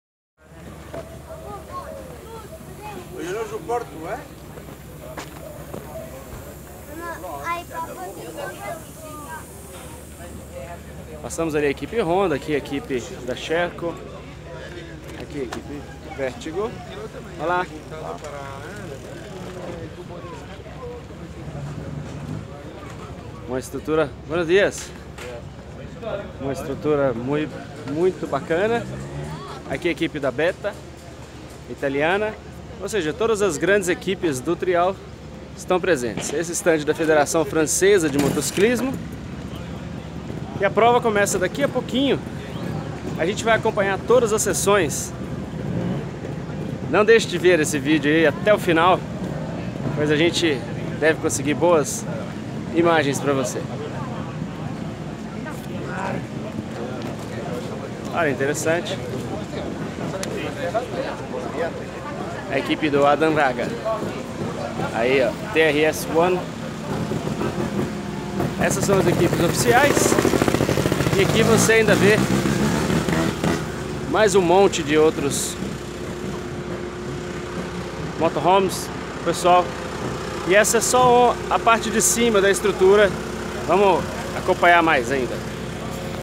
Mundial de Trial - Camprodon - vídeo 2
O Jeca Jóia foi acompanhar a etapa de abertura do Campeonato Mundial de Trial, com a presença de feras como Toni Bou, Cabestani, Adam Raga, Fujinami, e muitos outros.